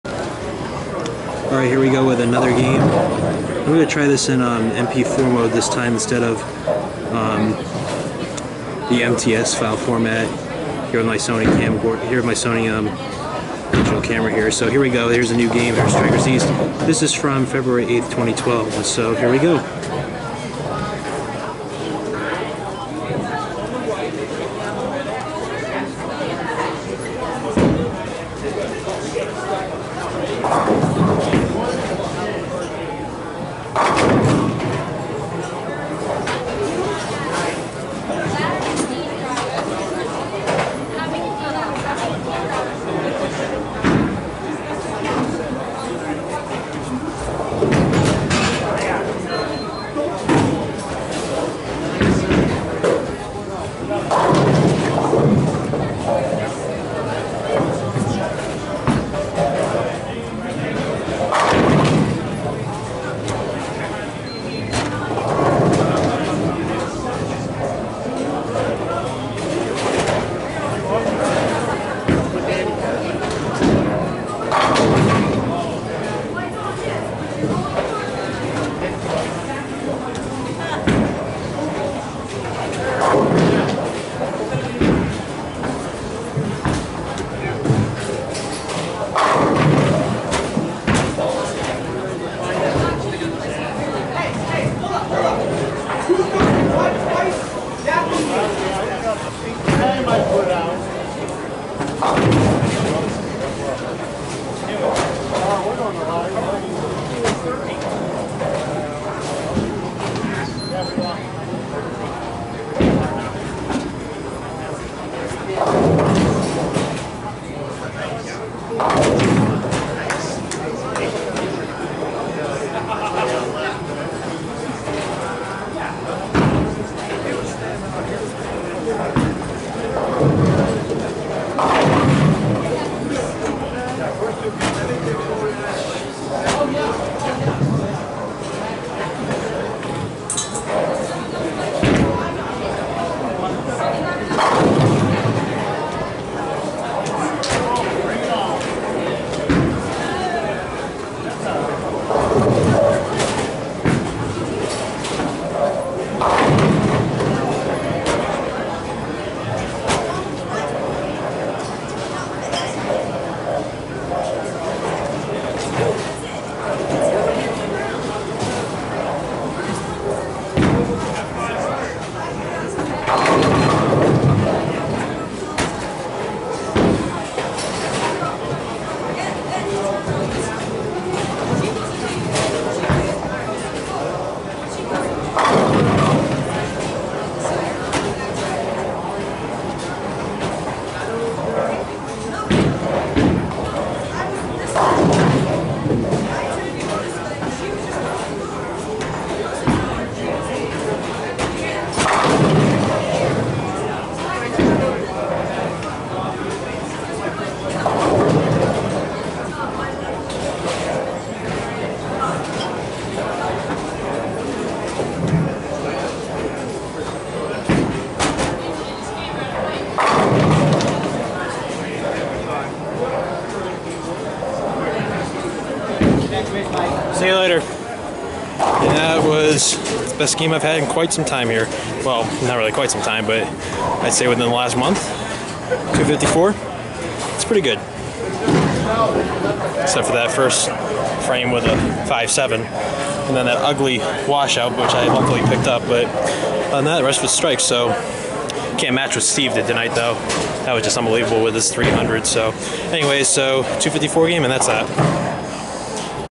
254 Game at Strikers East in Raymond, NH (From 2/8/12)
Here's a pretty good game I rolled in practice yesterday, especially since I had the thumb hole on my Black Widow re-drilled to an oval thumb hole.